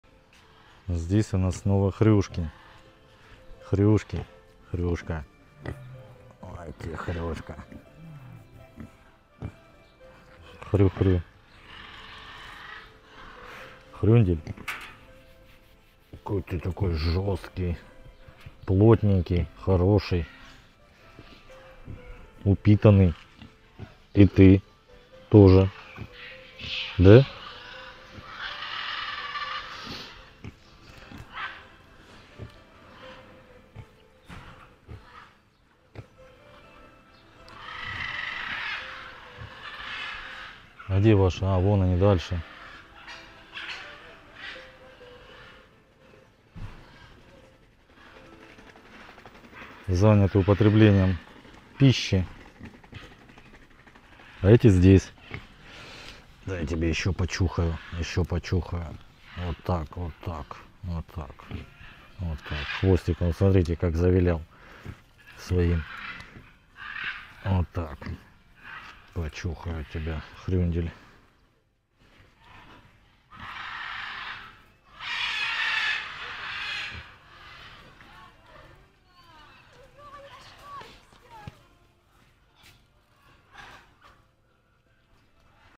ХРЮШКИ парка львов «Тайган» РУЧНЫЕ и ДОБРЫЕ
В парке львов "Тайган" живут ручные маленькие поросята, которых можно погладить и покормить.
Поддержать автора:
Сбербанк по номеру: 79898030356
Сбербанк-карта: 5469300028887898 Сергей Григорьевич М.
БУДЬТЕ СО МНОЙ ВЕЗДЕ!!!